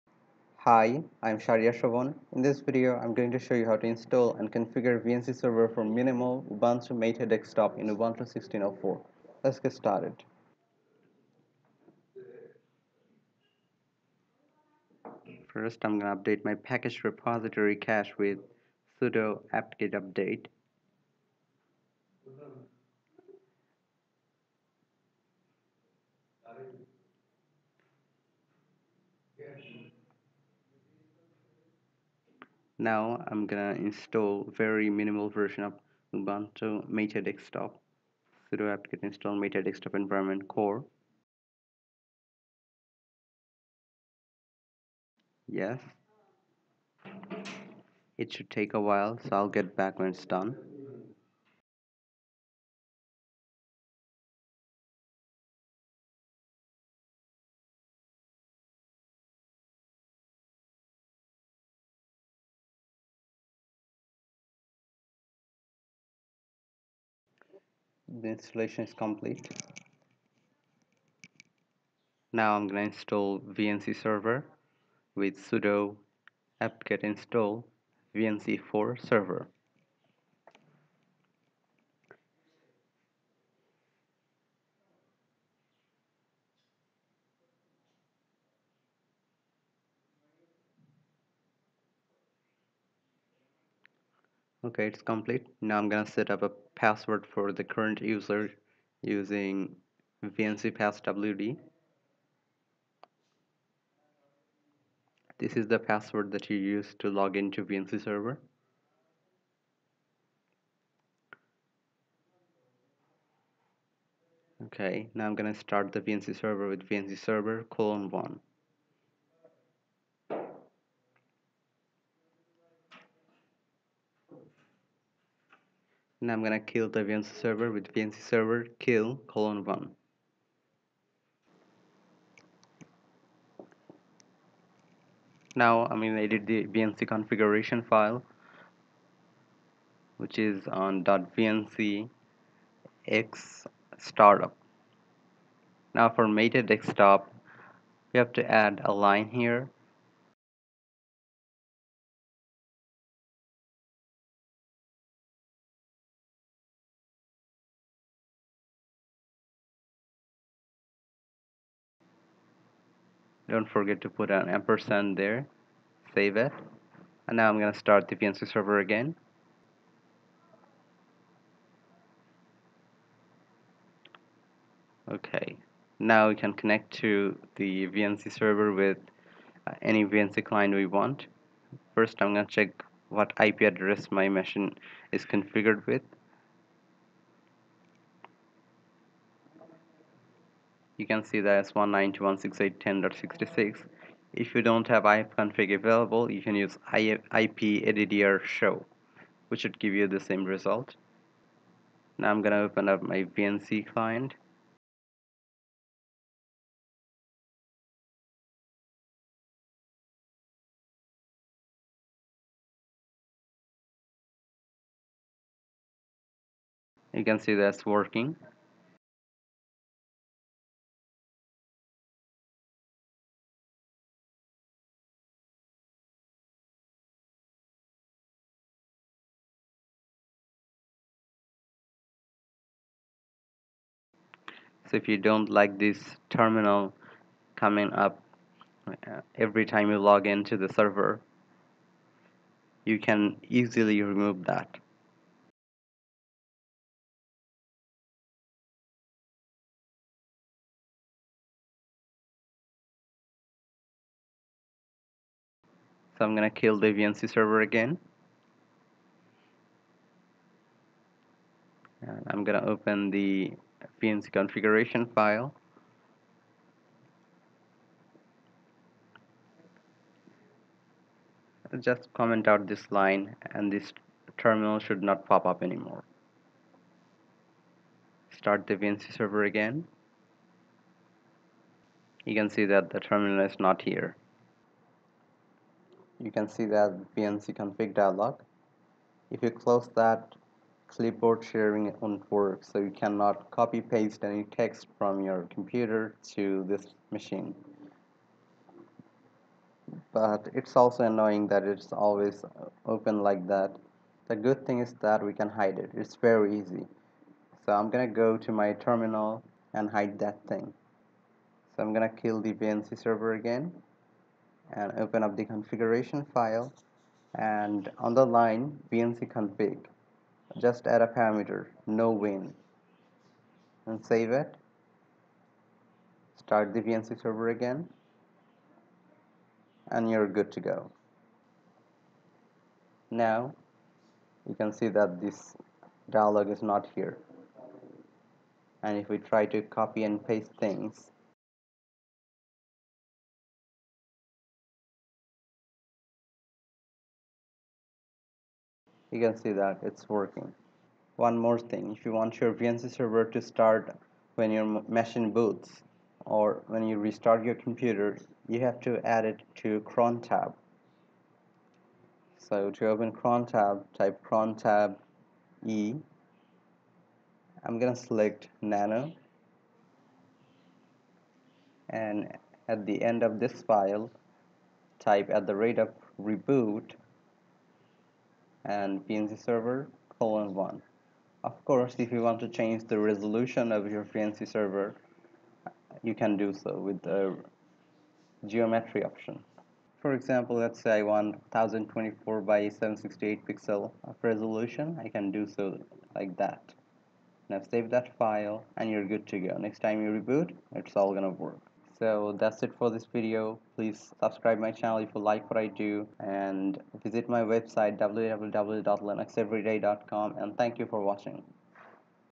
Install & Configure VNC Server for Minimal Ubuntu MATE Desktop in Ubuntu 16.04
In this video I demonstrated how to install and configure vnc server for minimal ubuntu mate desktop on Ubuntu 16.04. This tutorial should also work on Debian 9 stretch. I used vnc4server for this demo.

This video covers the following topics:
- Install Minimal Ubuntu MATE desktop on Ubuntu 16.04 LTS
- Install and Configure basic VNC Server on Minimal Ubuntu MATE Desktop
- disable automatic startup of terminal from vnc client
- hide vncconfig window from the panel
- configure clipboard share on vnc server
- automatically start vnc server on boot/reoot
- add vnc server to startup